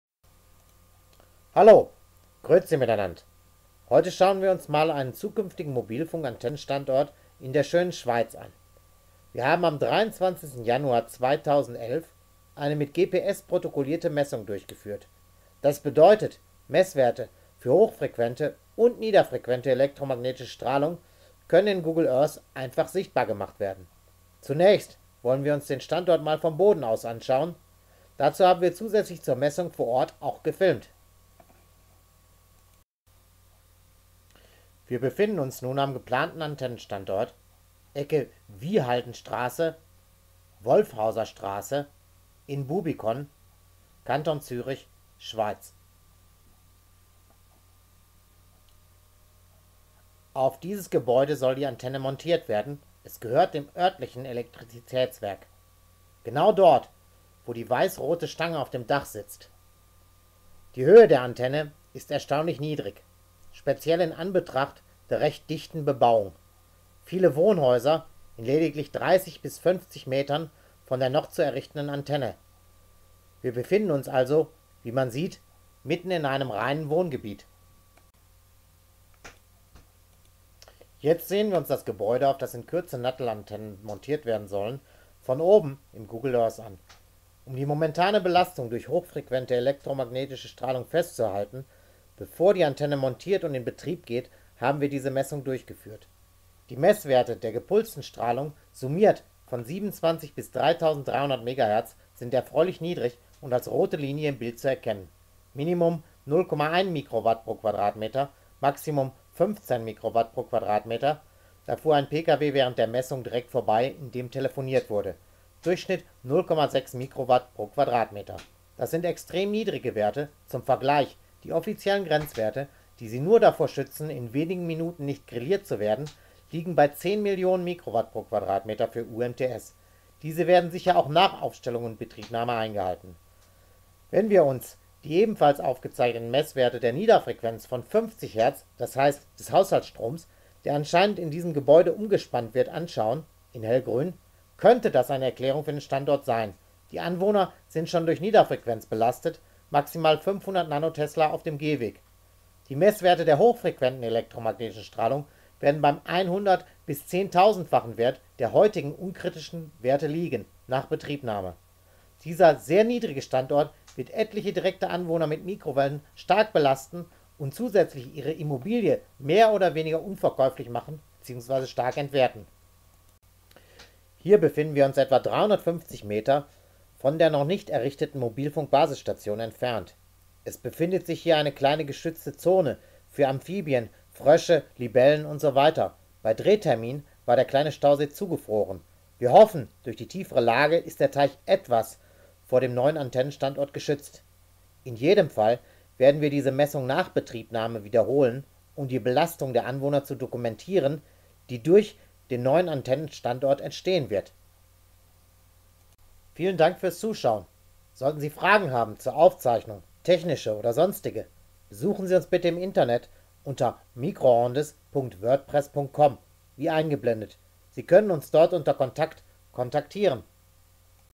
Messung zukünftiger Swisscom Antennen-Standort in Bubikon, Kanton Zürich, Schweiz
Die Messung wurde mit GPS protokolliert und mit Google-Earth im Video visualisiert! Der kurze Film (3:30 Minuten) zeigt die elektromagnetischen Felder vor Aufstellung einer Swisscom Antenne am zukünftigen Standort auf einem Gebäude der Elektrizitätsgenossenschaft Bubikon.